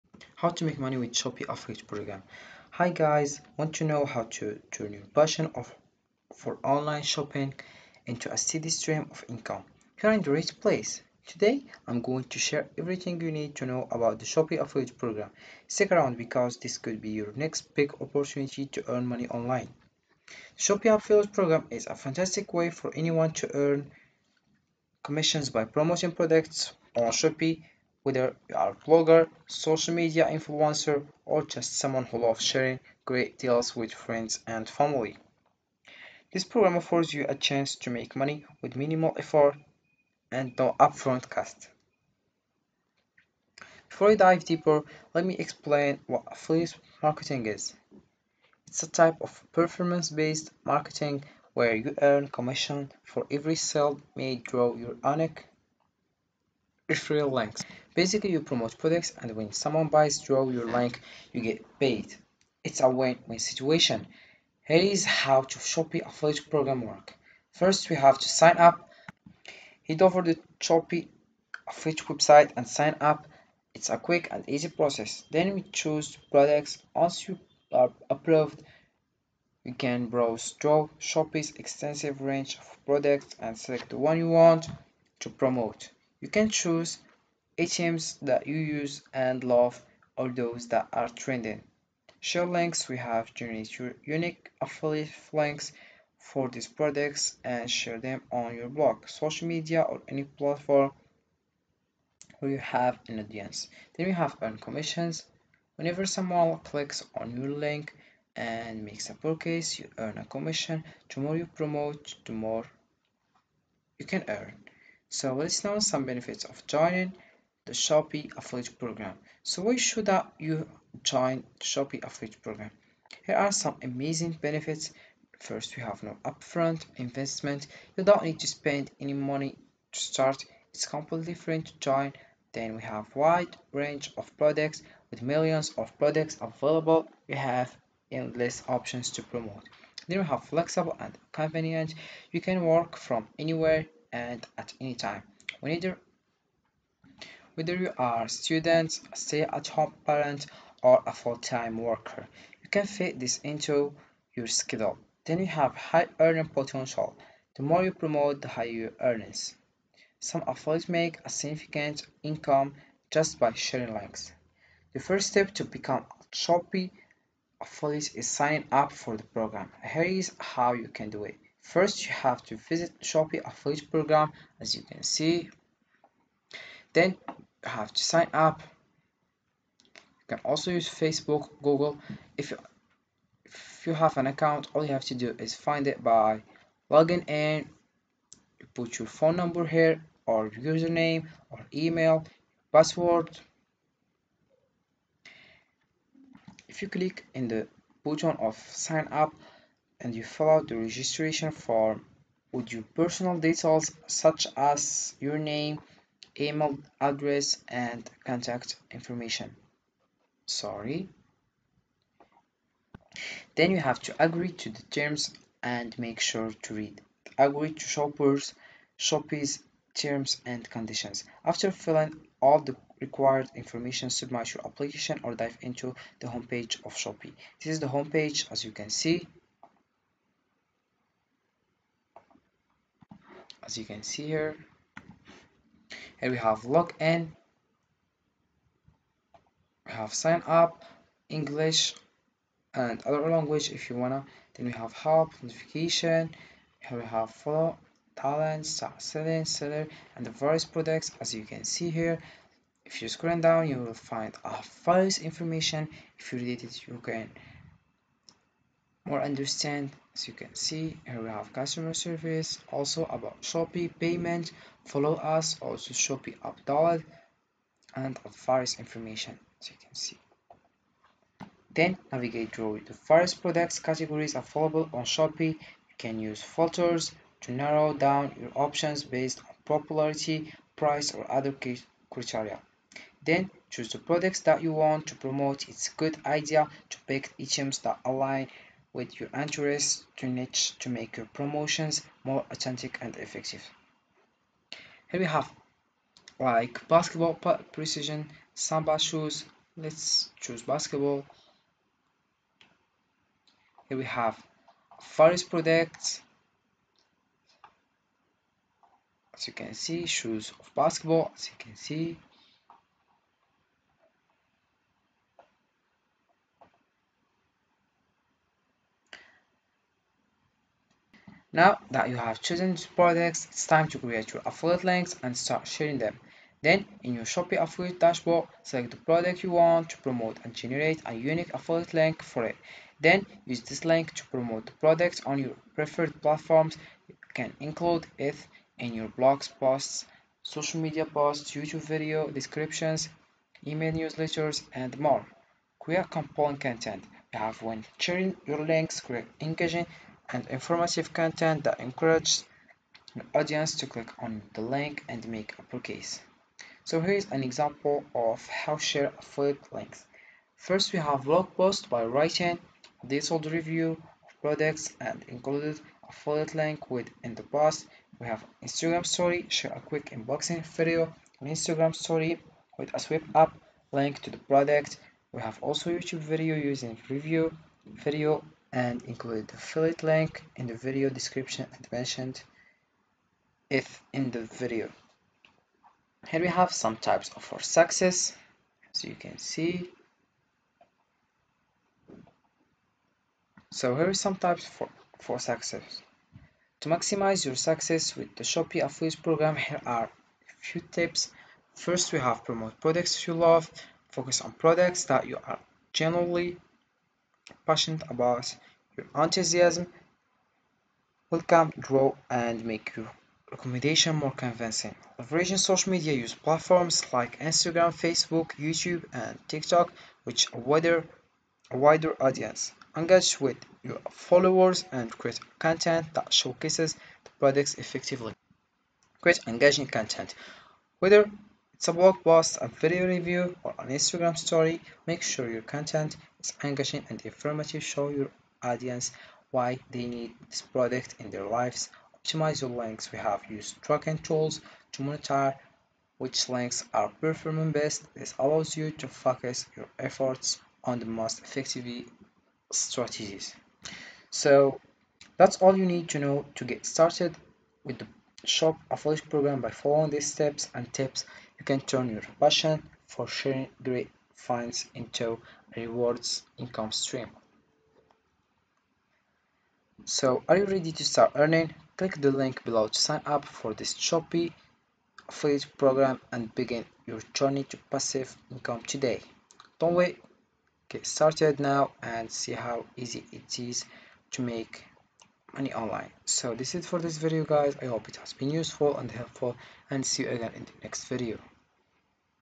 How to Make Money on Shopee: Insider Tips for Maximizing Earnings
in this video we discuss : Discover how to make money on Shopee with this easy guide! In this tutorial, we share proven strategies and tips to boost your income fast. Learn how to create effective listings, set competitive prices, and optimize your store for higher sales. Perfect for both beginners and experienced sellers, this step-by-step guide will help you maximize your profits effortlessly. Watch now and start boosting your Shopee income today!